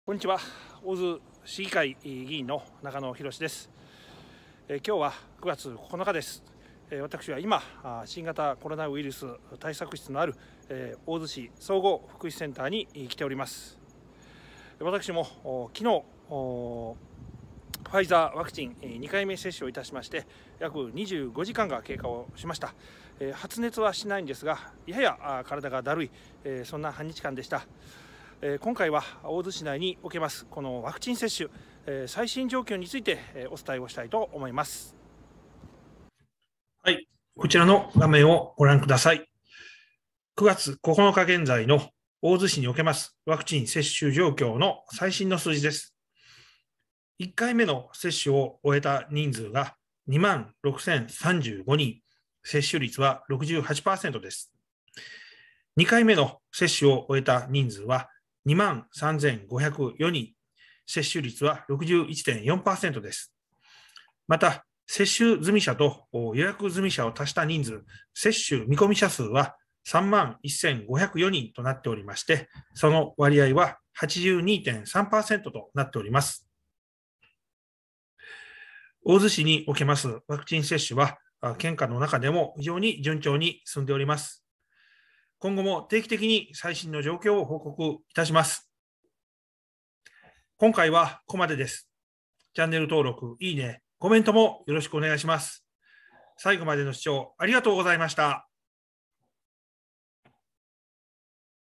#157 【大洲市議会議員】中野ひろし ワクチン接種済者数の最新状況は？？
ファイザーワクチン2回目を接種してから丸1日経過しました。今のところ、大きな副反応はありません。今回は大洲市内のワクチン接種済者数について、9/9現在の最新状況をお伝えします。